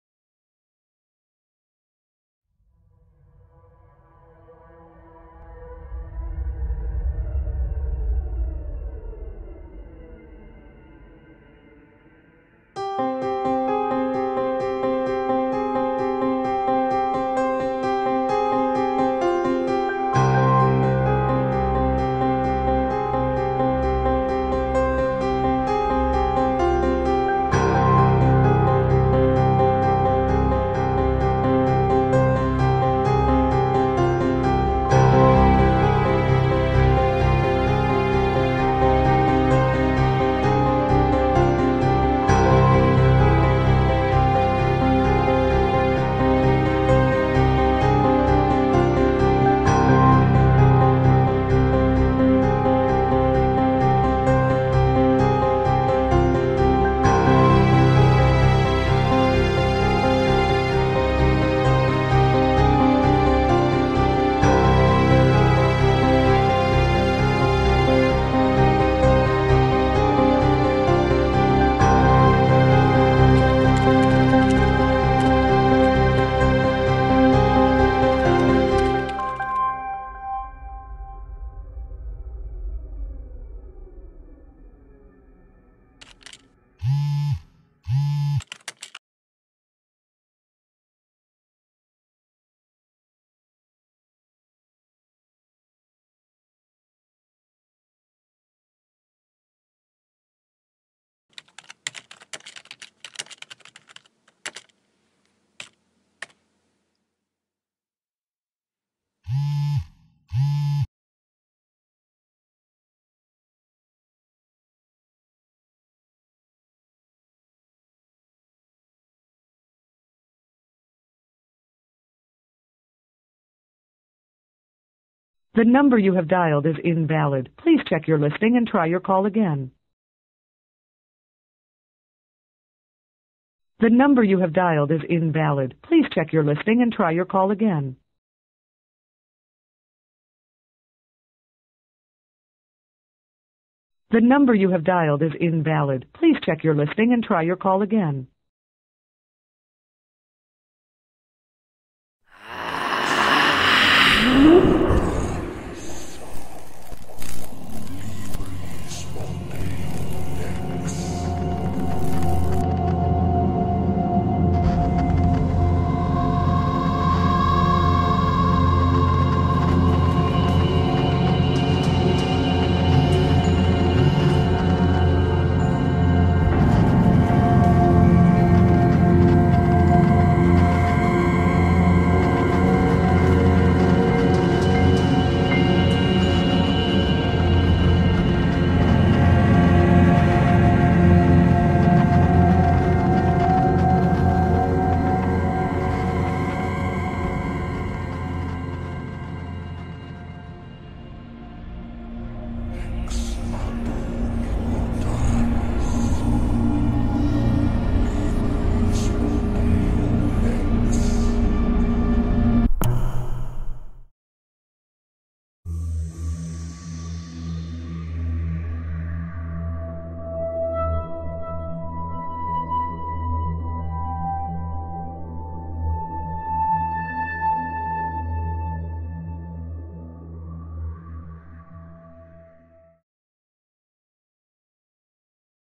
Spam - A narrative horror short feature film
Story, Screenplay, Cinematography, Camera work, Editing . . . everything was done by me in 7 hrs for a competition submission.